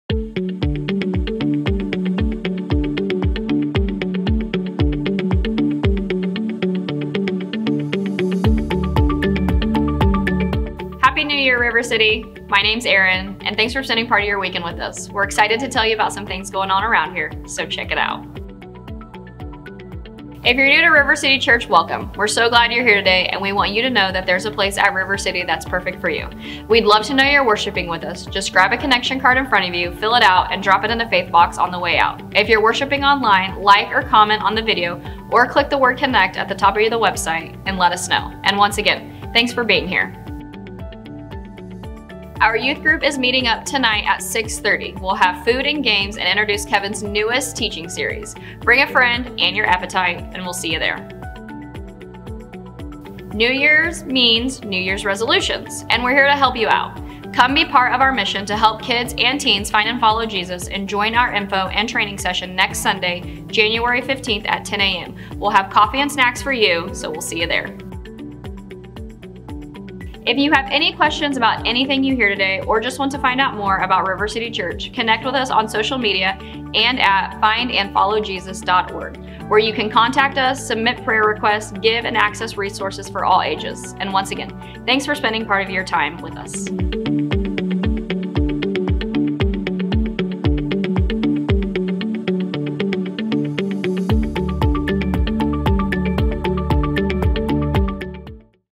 2021-01-09 Announcements
Missed our announcements this week? Want to rewatch and get those details you missed the first time? Watch this week's announcements right here!

Our youth group is meeting up tonight at 6:30! We’ll have food and games and introduce Kevin’s newest teaching series. Bring a friend and your appetite and we’ll see you there!

New year means new resolutions and we’re here to help you out! Come be part of our mission to help kids and teens find and follow Jesus, and join our info and training session next Saturday—January 15—at 10 AM. We’ll have coffee and snacks for you, so we’ll see you there!